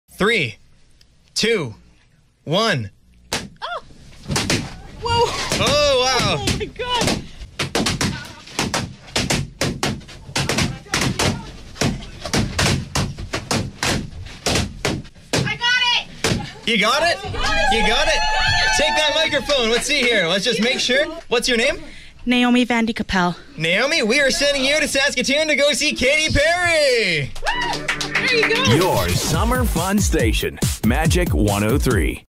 Pop For Perry | Short Version
Short version of August's "Pop for Perry" contest used for a company presentation.